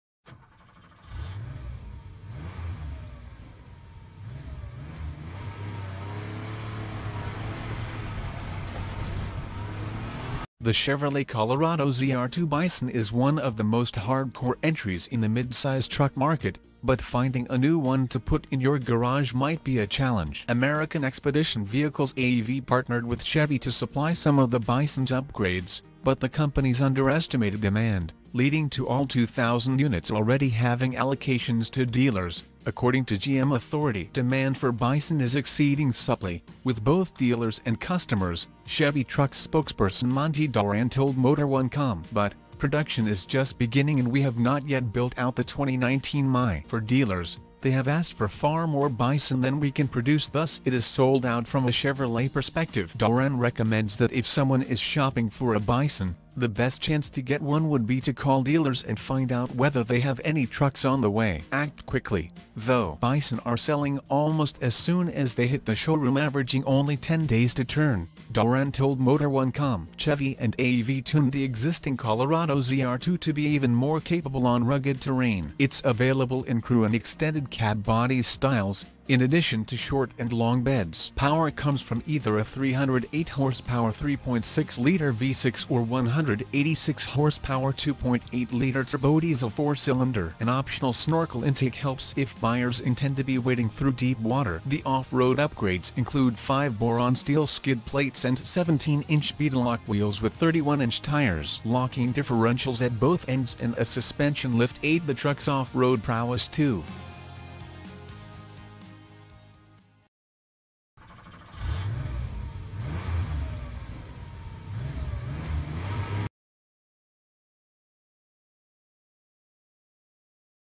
[Hot news] Chevy Colorado ZR2 Bison Demand Beats Bowtie's Expectations
The Chevrolet Colorado ZR2 Bison is one of the most hardcore entries in the midsize truck market, but finding a new one to put in your garage might be a challenge. American Expedition Vehicles (AEV) partnered with Chevy to supply some of the Bison's upgrades, but the companies underestimated demand, leading to all 2,000 units already having allocations to dealers, according to GM Authority.